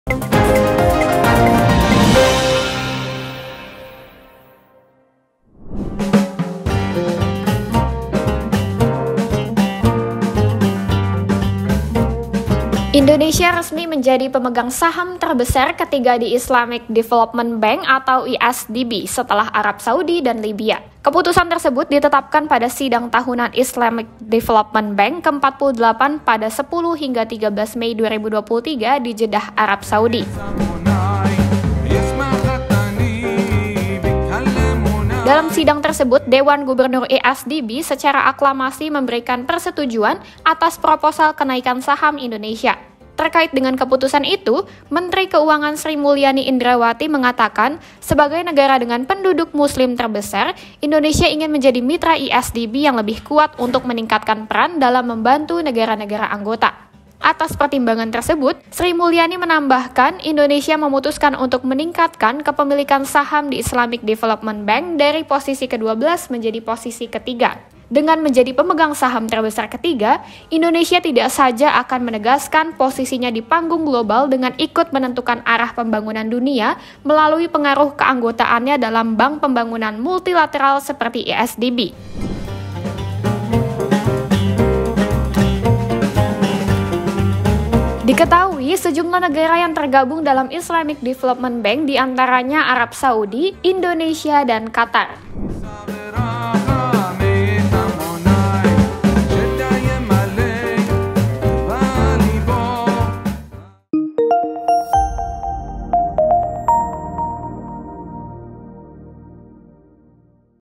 EKONOMI || RI Jadi Pemegang Saham Terbesar Ketiga di Islamic Development Bank
Indonesia resmi menjagi pemegang saham terbesar ketiga di Islamic Development  Bank (IsDB) setelah Arab Saudi dan Libya. Keputusan tersebut ditetapkan  pada Sidang Tahunan Islamic Development Bank  ke-48 pada 10-13 Mei 2023 di Jeddah, Arab Saudi. Dalam sidang tersebut Dewan Gubernur IsDB secara aklamasi memberikan persetujuan atas proposal kenaikan saham Indonesia.
Terkait dengan keputusan itu, Menteri Keuangan (Menkeu)  Sri Mulyani Indrawati mengatakan, sebagai negara dengan penduduk muslim terbesar, Indonesia ingin menjadi mitra IsDB yang lebih kuat untuk meningkatkan peran dalam membantu negara-negara anggota.
Atas pertimbangan tersebut, Sri Mulyani menambahkan, Indonesia memutuskan untuk meningkatkan kepemilikan saham di Islamic Development Bank dari posisi ke-12 menjadi posisi ke-3.
Dengan menjadi pemegang saham terbesar ketiga, Indonesia tidak saja akan menegaskan posisinya di panggung global dengan ikut menentukan arah pembangunan dunia melalui pengaruh keanggotaannya dalam bank pembangunan multilateral seperti IsDB.
Diketahui sejumlah negara yang tergabung dalam Islamic Development Bank diantaranya Arab Saudi, Indonesia dan Qatar